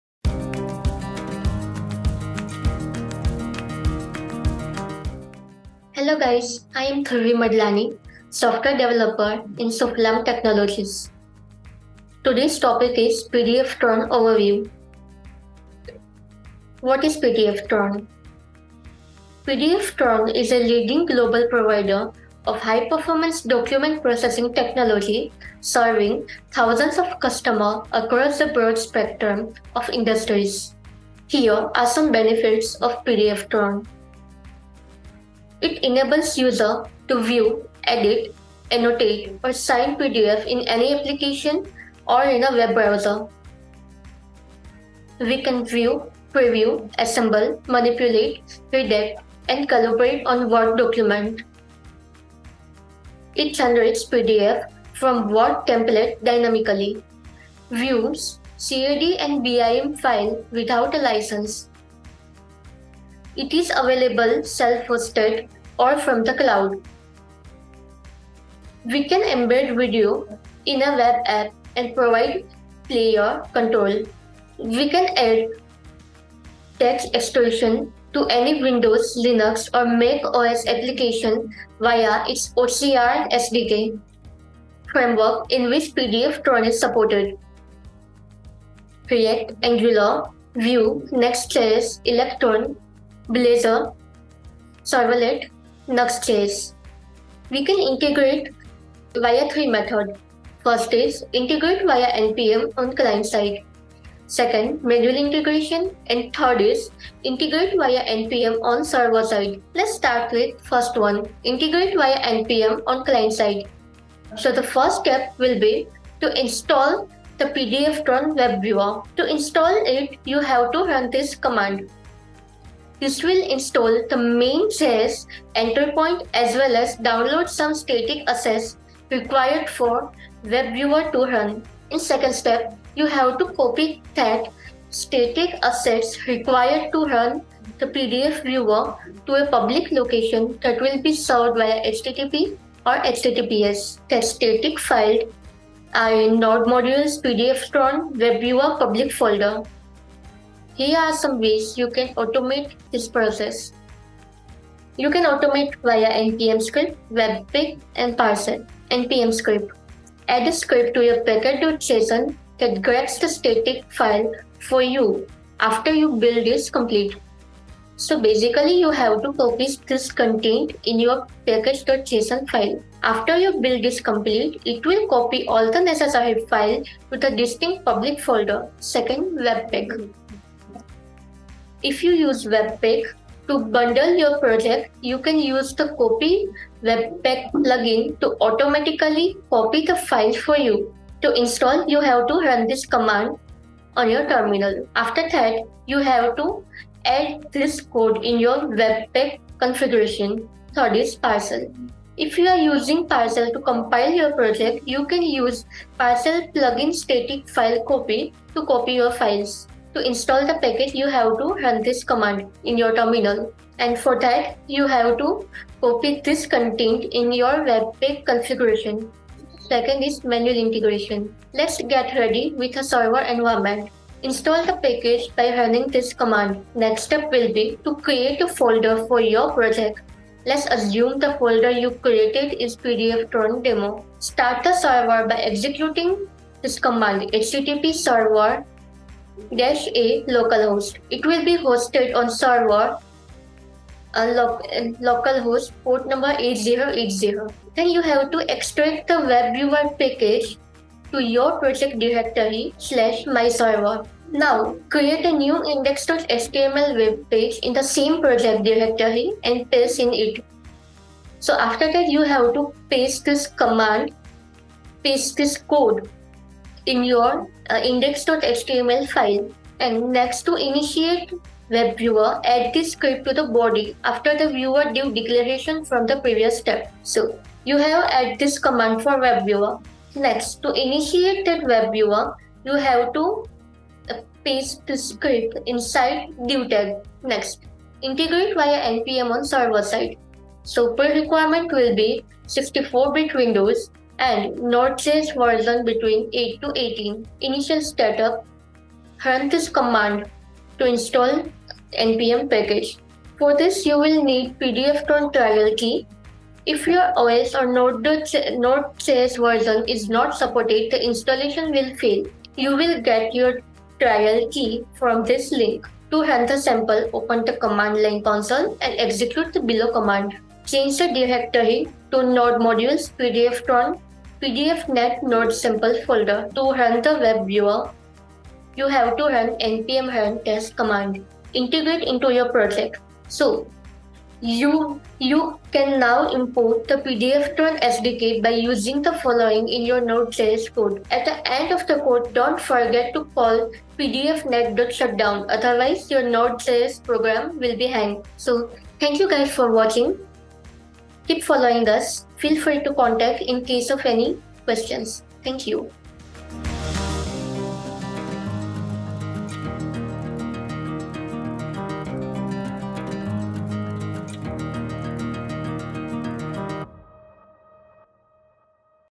What is PDFTRON ? | Explained in Just 6 Minutes | Sufalam Technologies
An overview of PDFTRON. Let us learn about its benefits and integration in our informative video.